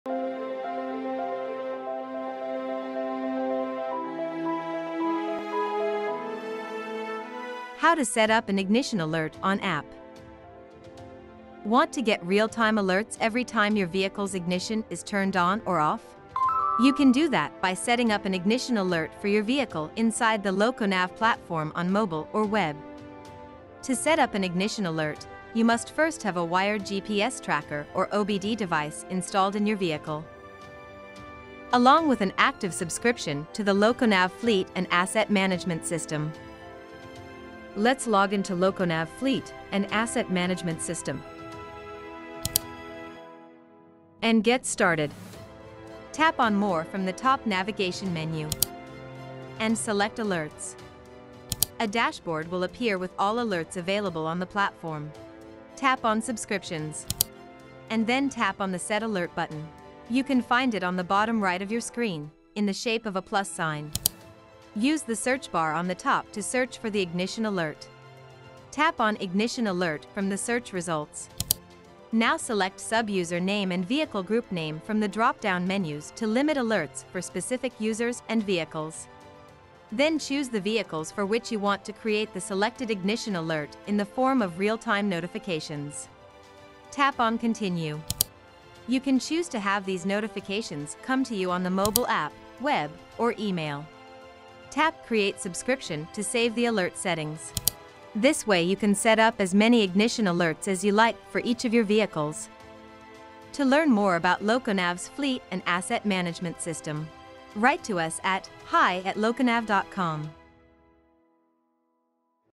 LocoNav TrainingGuides | How to Set Up Ignition Alerts (for App)
Did you know? You can get real-time notifications every time your vehicle ignition turns on and off.

What do you need to get started?
- A wired GPS Tracker or an On-board diagnostics (OBD) device installed in your vehicle.
- An active subscription to LocoNav Fleet & Asset Management System.

How to set up Ignition alerts?
Step 1: Tap on ‘More’ from the top navigation menu and select ‘Alerts’. A dashboard will appear with all alerts available on the platform.
Step 2: Tap on ‘Subscriptions’ and then tap on the ‘Set Alert’ button. You can find it on the bottom-right of your screen, in the shape of a plus sign.
Step 3: Use the Search bar on the top to search for the ‘Ignition’ alert.
Step 4: Tap on ‘Ignition Alert’ from the search results.
Step 5: Now select Sub User Name and Vehicle Group Name from the dropdown menus to limit alerts for specific users and vehicles.
Step 6: Then choose the Vehicles for which you want to create the selected Ignition Alert in the form of real-time notifications.
Step 7: Tap on ‘Continue’.
Step 8: You can choose to have these notifications come to you on the mobile app, web, or email.
Step 9: Tap ‘Create Subscription’ to save the alert settings. This way you can set up as many ignition alerts as you like for each of your vehicles.